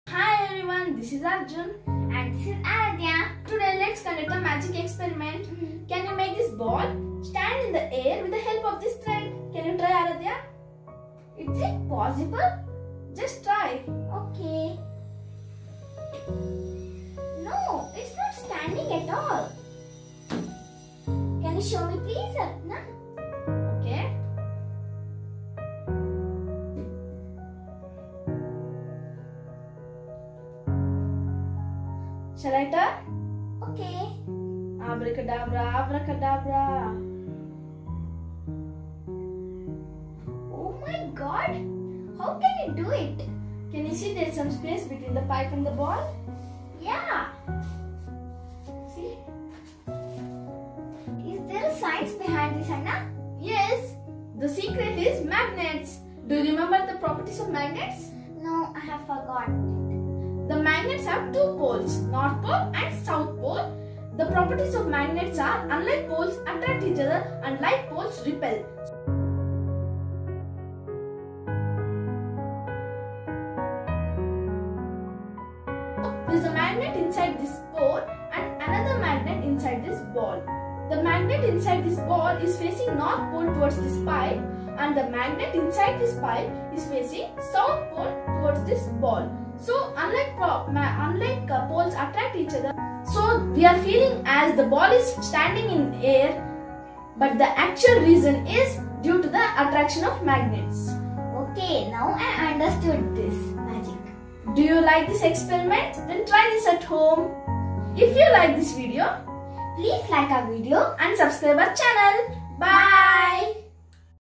Amazing SCIENTIFIC Experiment at home 2021 || "How can the ball stand on the thread" || Must watch
This is a wonderful experiment for kids during lockdown
This experiment helps to learn the properties of magnets
this experiment develops scientific attitude among the children